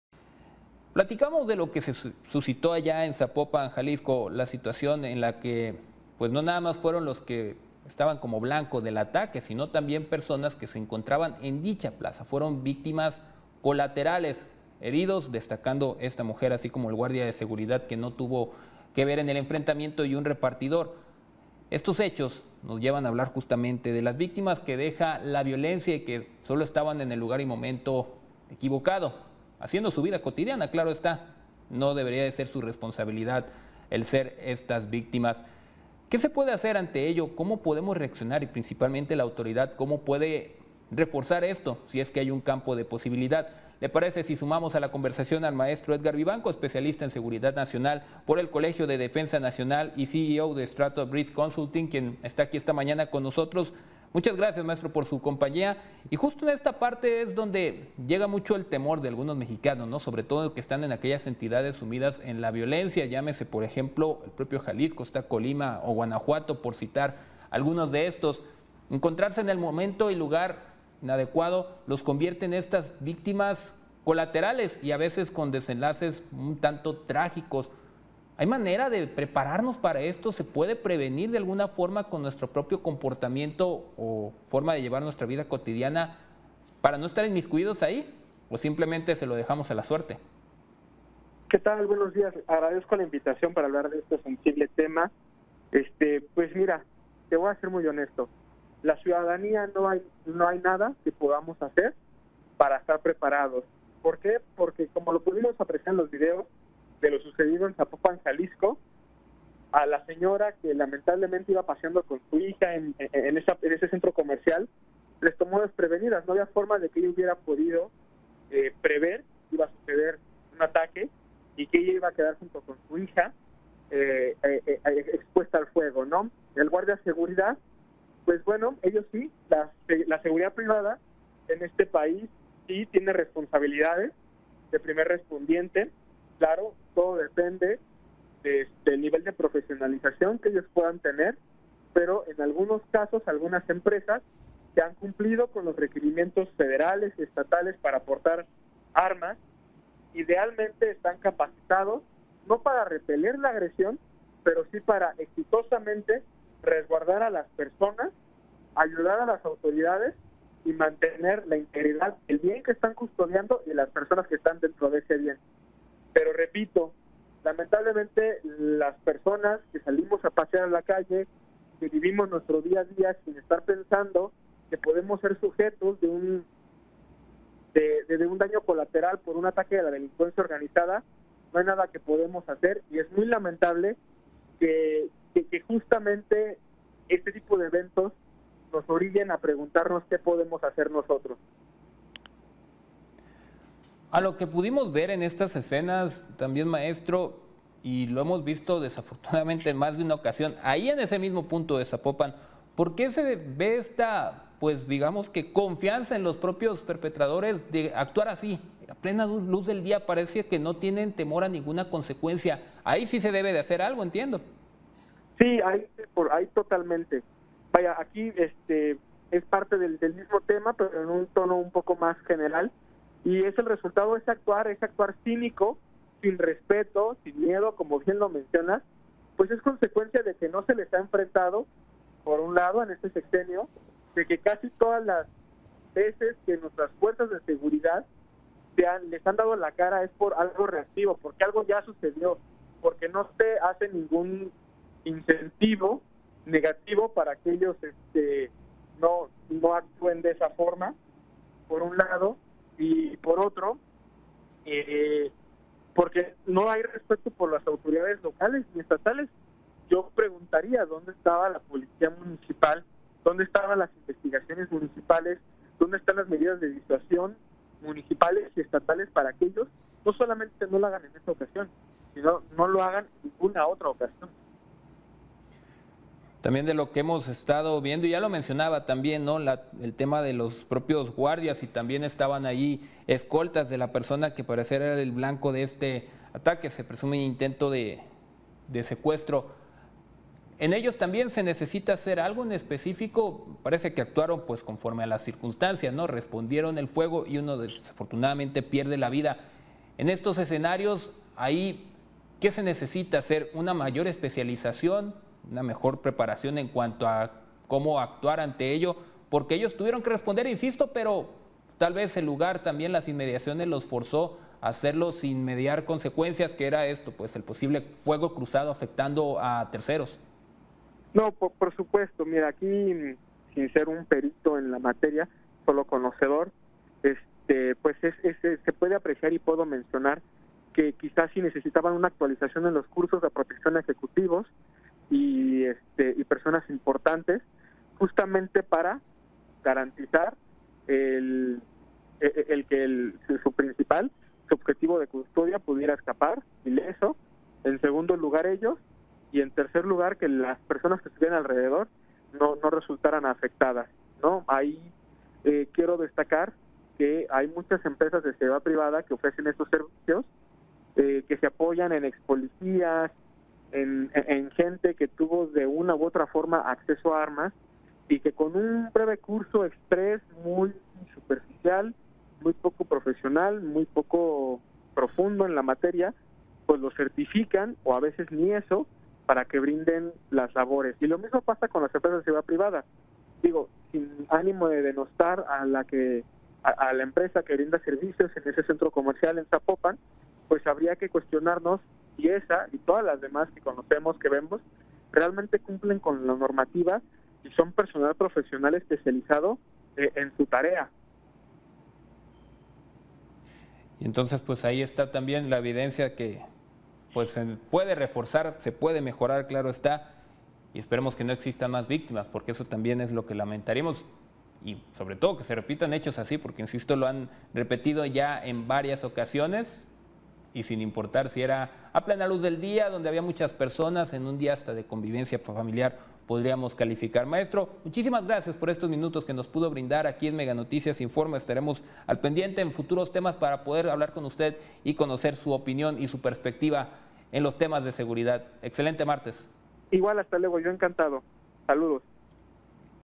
Víctimas colaterales de la violencia
Francisco Hernández en entrevista con el Mtro. Edgar Vivanco, Especialista en Seguridad Nacional por el Colegio de Defensa Nacional y CEO de STRATOP RISK CONSULTING